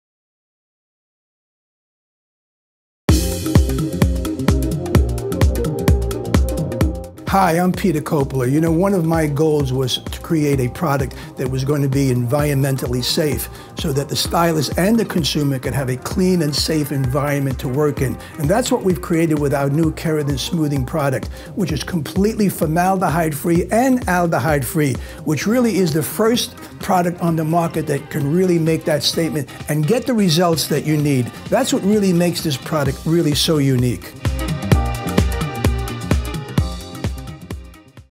Peter Coppola Keratin Smoothing Treatment
Peter Coppola introduces his new Keratin Treatment which is Formaldehyde-free and Aldehyde-free and does not contain contain dangerous chemicals found in other Keratin hair treatments. The new Keratin Smoothing Treatment formula uses the highest quality ingredients combined with a low pH mechanism to soften and smooth every hair type without the concern of harmful chemicals. Infused with vital Keratin amino acids and damage-reversing Ceramides. It also provides the ultimate anti-aging boost transforming the hair and smoothing frizz for a minimum of 3 months.

This revolutionary keratin treatment protects against the most harmful environmental stressors including: over-processing from coloring or chemical services, heat styling, UV exposure and daily styling damage for stronger, smoother, and age-defying results, leaving your client with smooth, frizz free hair.